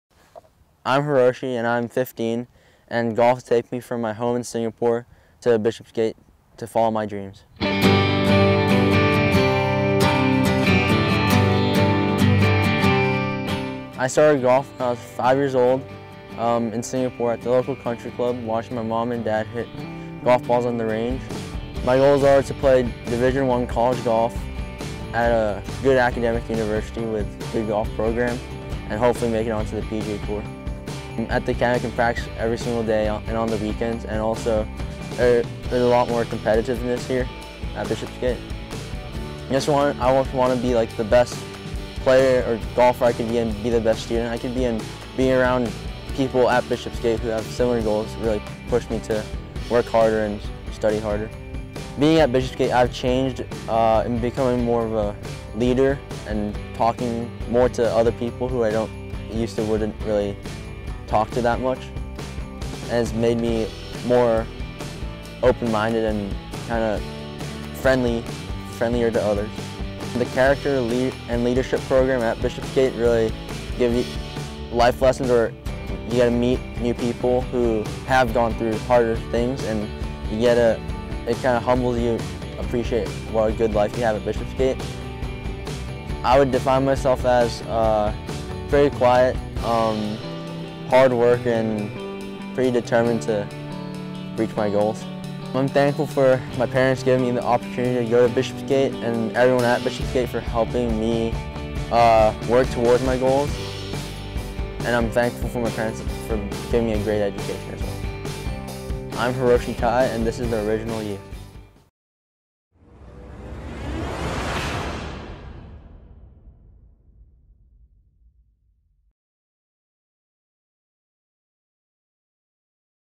The Original You: Hiroshi Tai
BGGA student Hiroshi Tai talks about how he got started in golf and the impact the Academy has had on his life.